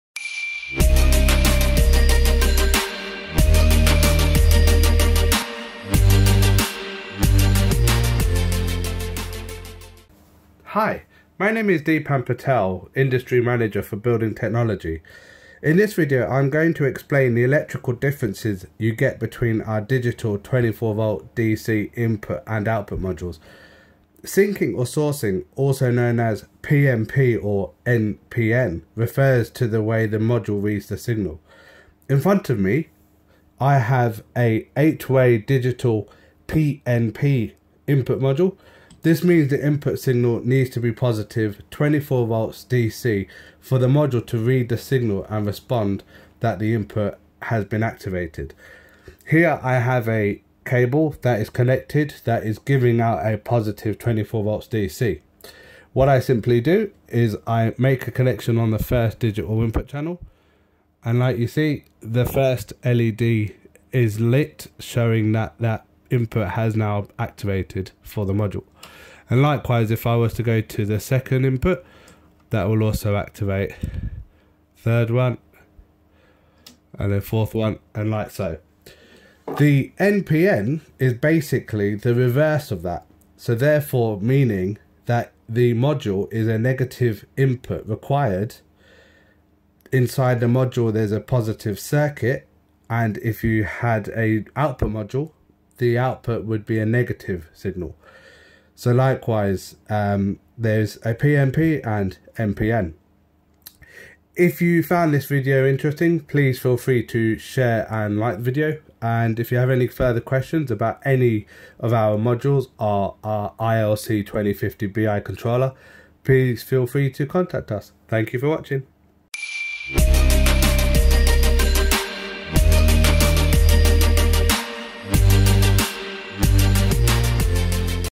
ILC 2050 BI Controller- The difference between NPN and PNP
Deepan explains the electrical differences you can get between a digital 24 V DC Input and Output modules for the ILC 2050 BI Controller.